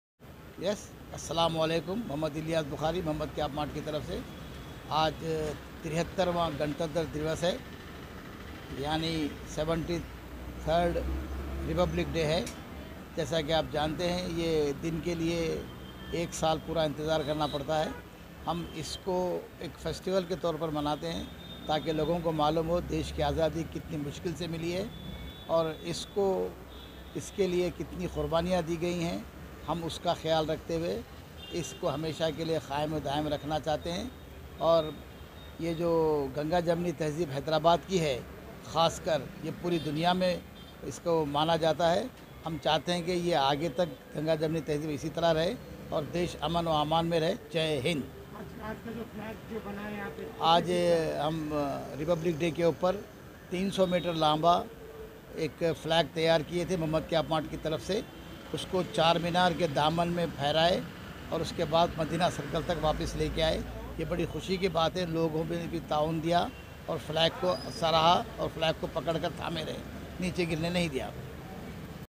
Hyderabad MP (MIM) party president Asaduddin Owaisi unveiled the national flag at Medina X Road in
Hyderabad ..

 Old town

 Medina

 Hyderabad MP (MIM) party president Asaduddin Owaisi unveiled the national flag at Medina X Road in Patna on the occasion of the 73rd Republic Day.

  The Pattar Gatti corporator involved was several MIM leaders.